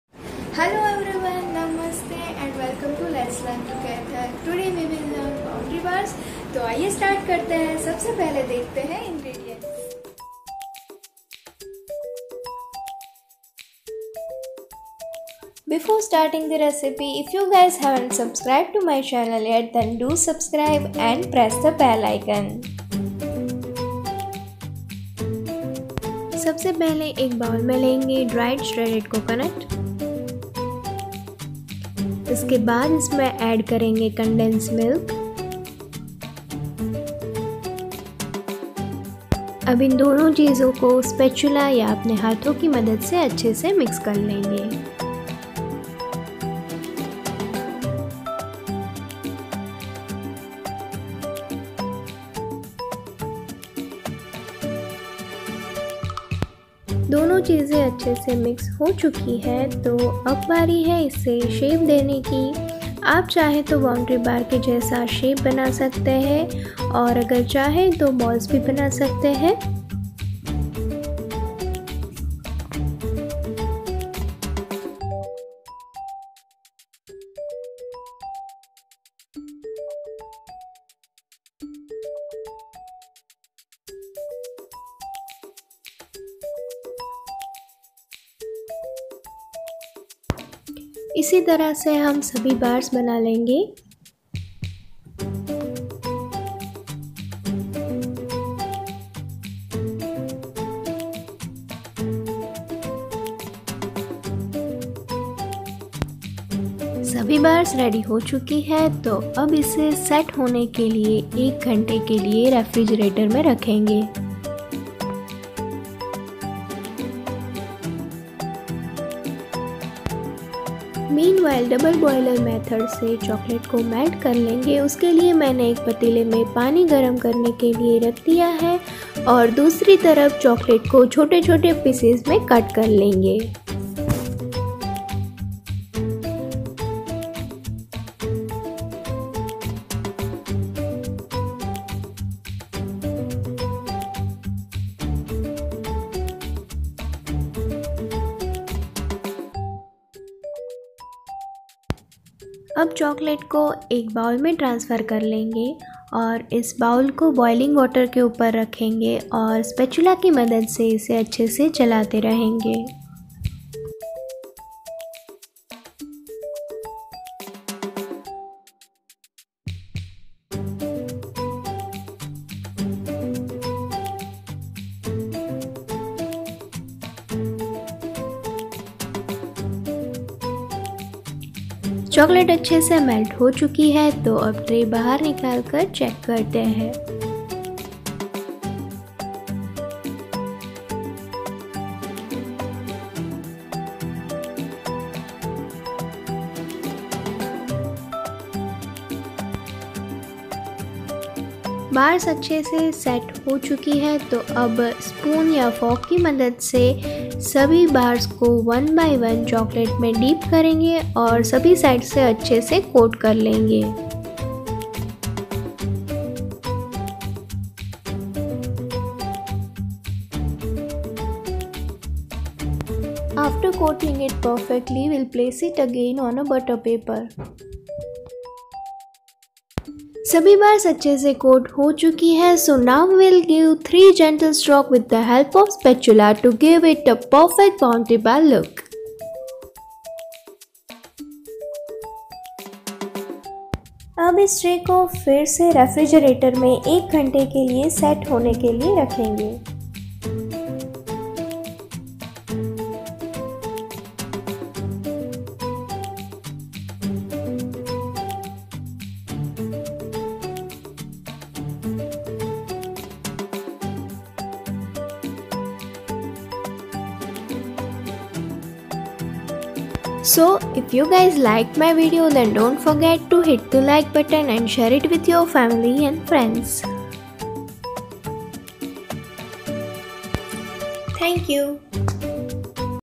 Bounty Bar Recipe | 3 Ingredients Recipe | ANERI N THAKKAR
Recipe - Bounty Bars

Ingredients

250gm Dry Shredded Coconut
200 gm Milk Compound
100 gm Condense milk

Do try in your Kitchen
And let me know how it turned Out !!!